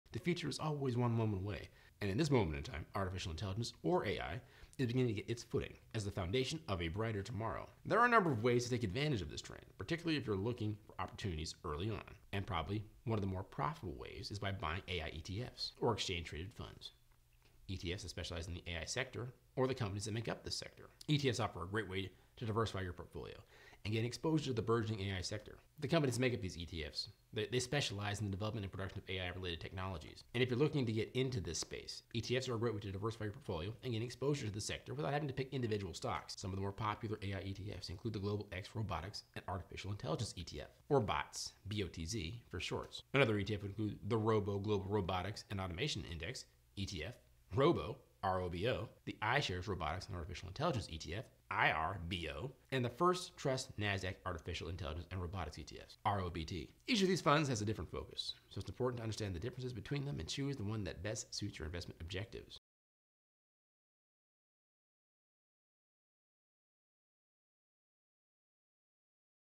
"AI Investing: Uncover the ETFs You NEED to Know About!"
In this video, we're going to be discussing some of the best ETFs for investing in AI, as well as giving you some tips on how to invest in AI.

If you're interested in getting started with AI investing, then this is the video for you! We'll discuss some of the most popular AI ETFs and give you tips on how to invest in them. We'll also discuss some of the risks and benefits of investing in AI, so be sure to watch this video to learn more about the benefits and risks of investing in AI!
AI ETFs to Trade